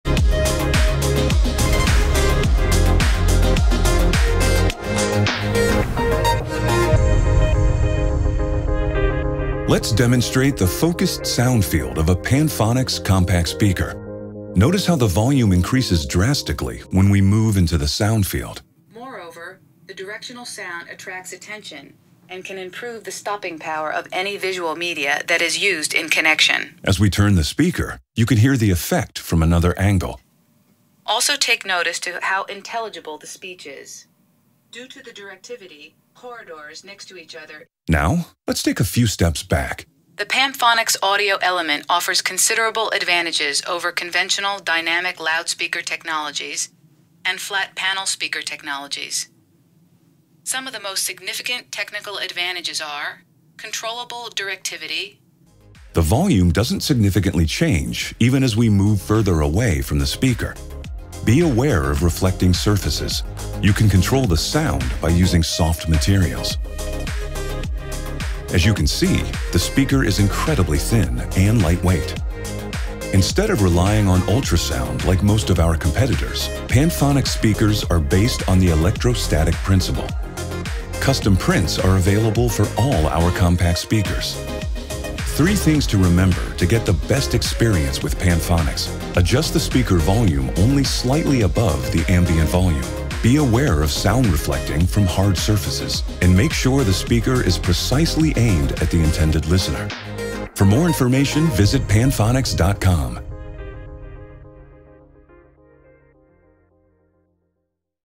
Ultrathin Directional Loudspeaker - Panphonics
A quick demonstration of a directional Panphonics COMPACT loudspeaker.

COMPACT features:

* Incredibly thin (20 mm) and lightweight (1,8 kg)
* Custom prints
* Like all of our products, doesn't use ultrasound